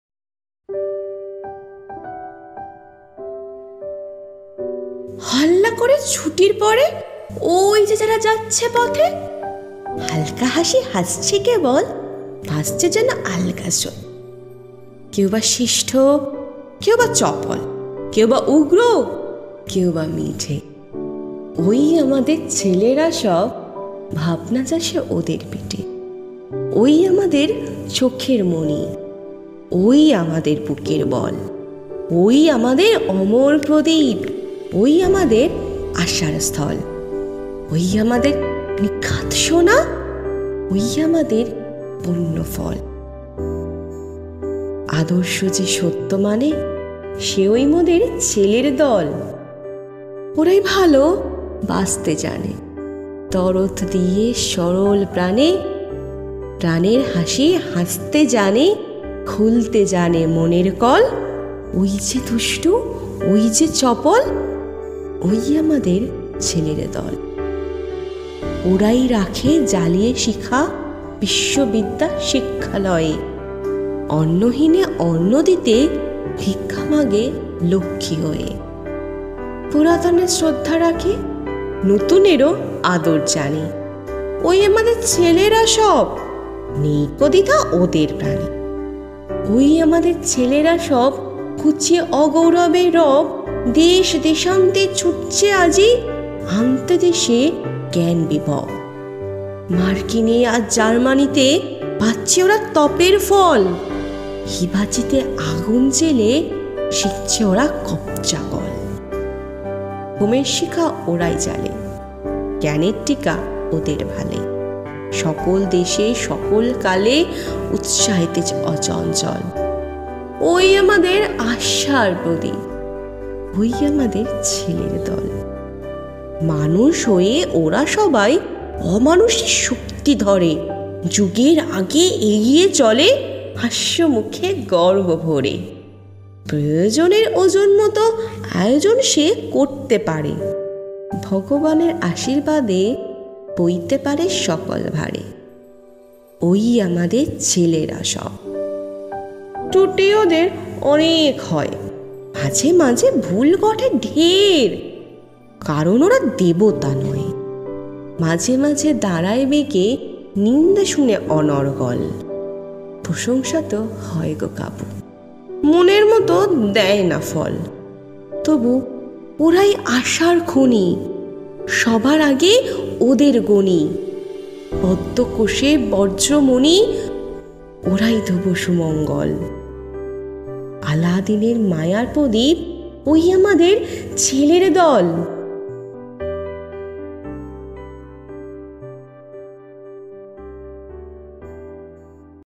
KagojKolomAudioSeries | CHELER DOL | SATYENDRANATH DUTTA
Kagoj Kolom Seba Songothon presents Satyendranath Dutta's CHELER DOL  at Kagoj Kolom Audio Series

Recited by Parnali Dutta
Music and Editing - Aniket